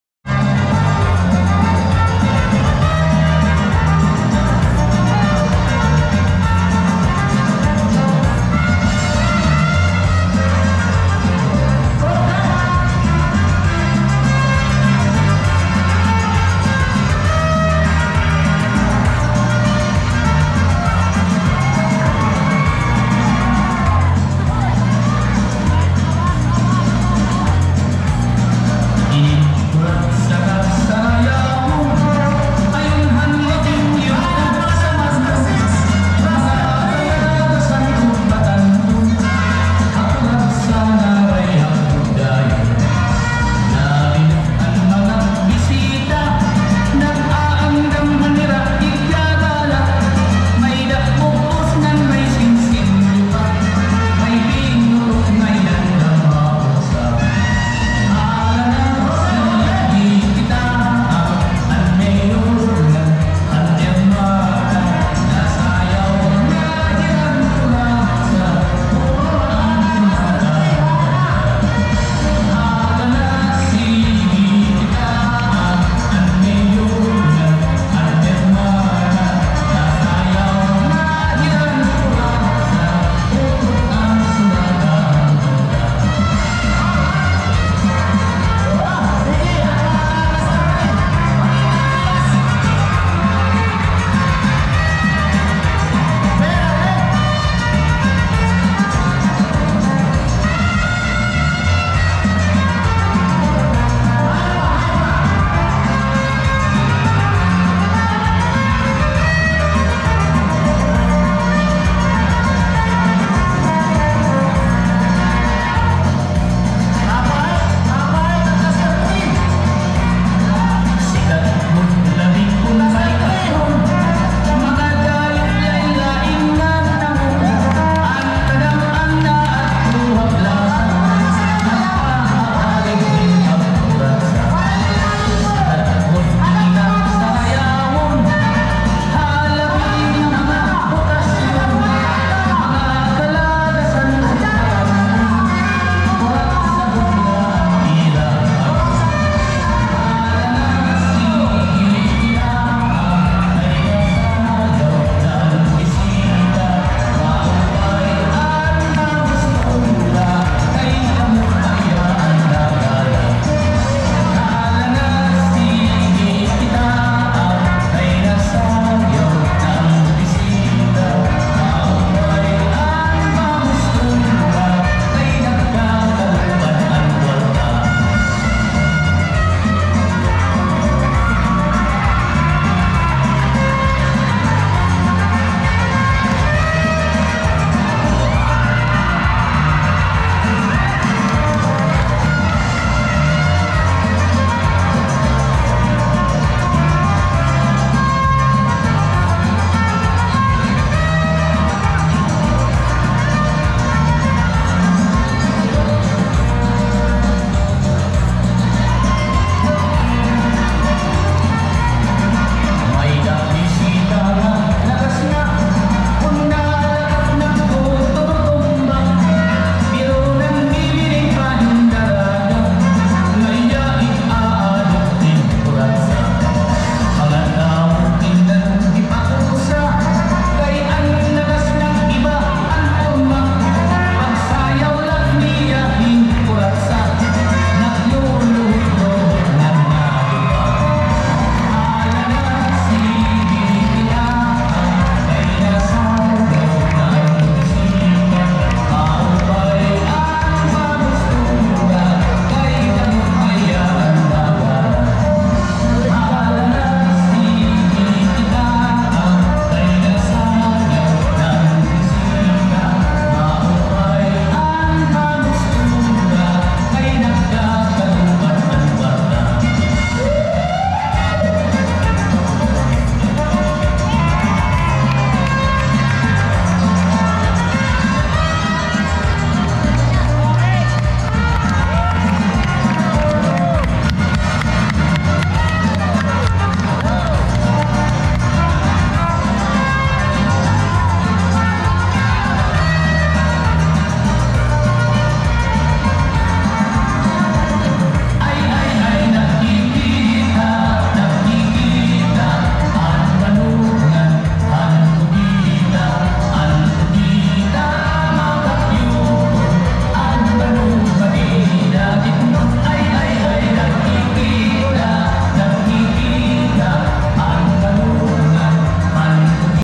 NAGLILIPARAN ANG PERA | NEW HOPE VILLAGE KURATSA DANCE 2022 FIESTA
Grabe ang Pera ng mga Waray NAGLILIPARAN.. Panoorin hanggang sa dulo..